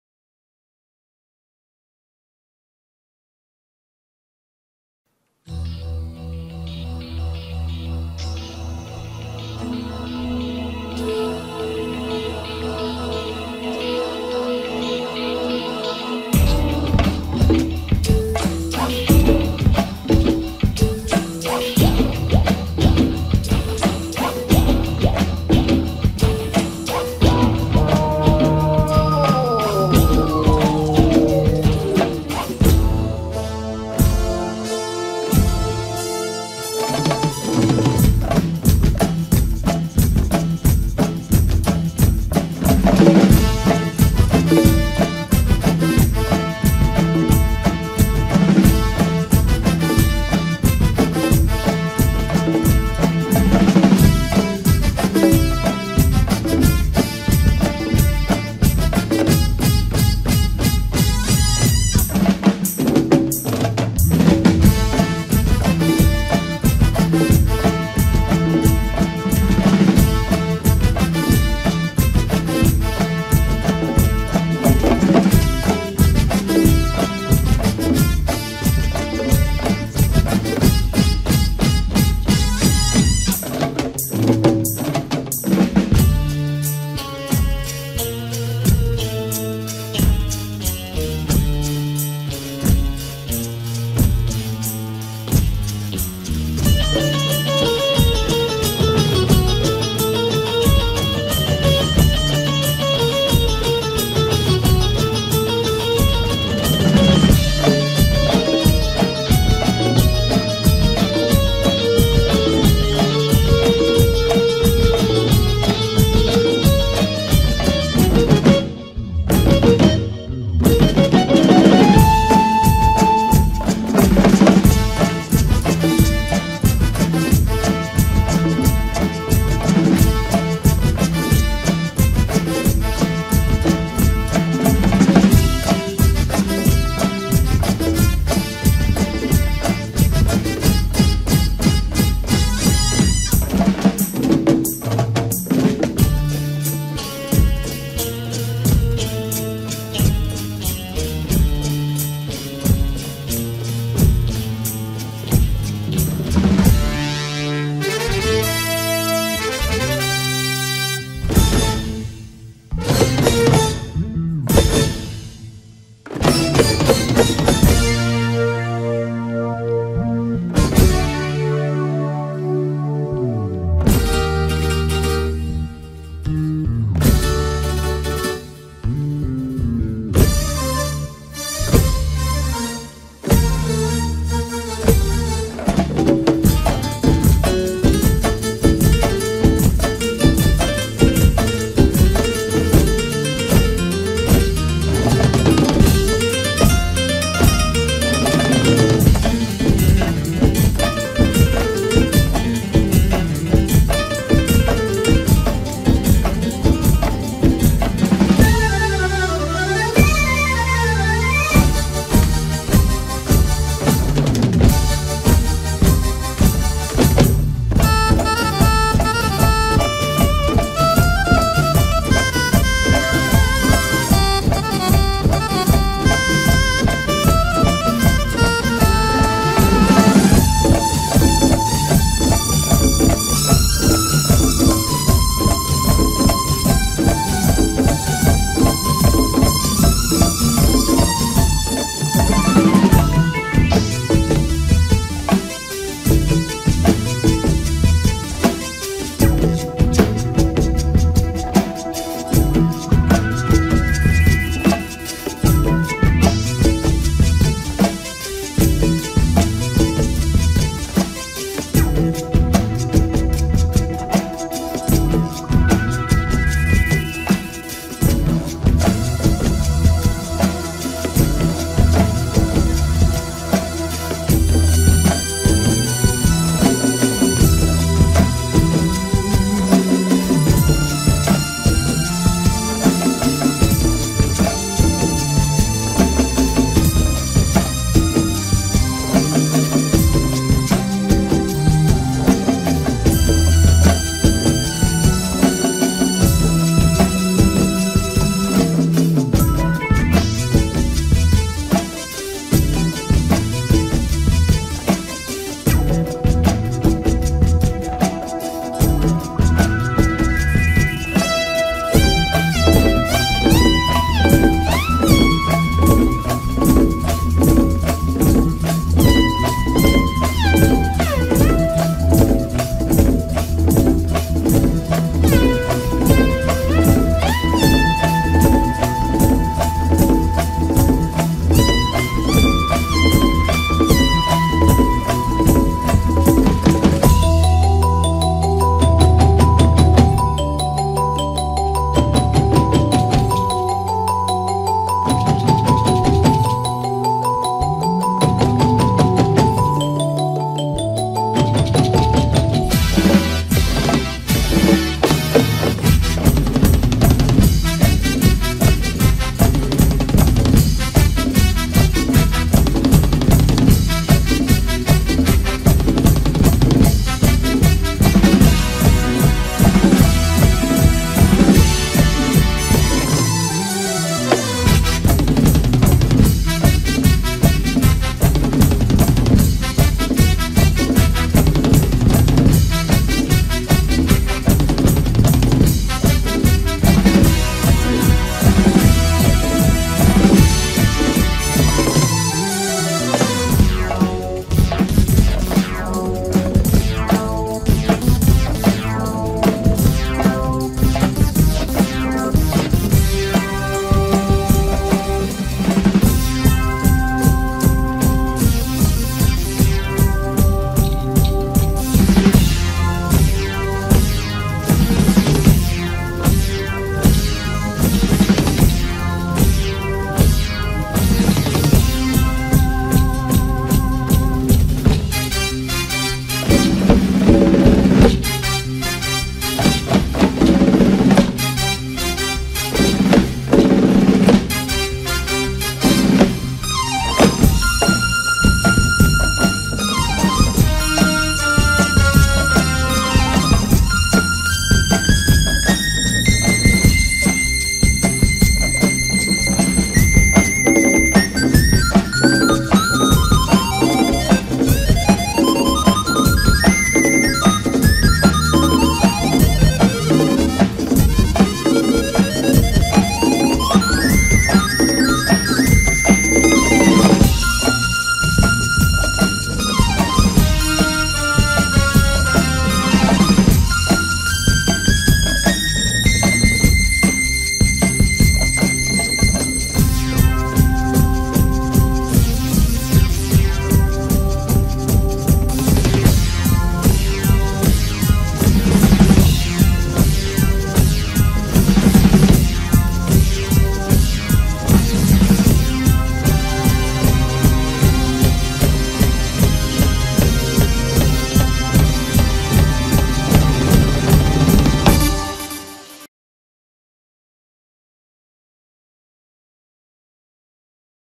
Band Rhythm Pulze - RDX With Generation Next
Students of Rhythm pulze Acedamy performing on themes of R d burman in Annual show 2024 in Ahmedabad  learn drums and percussion join Rhythm pulze contact Akul Raval on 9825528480 thanks vishv shah for video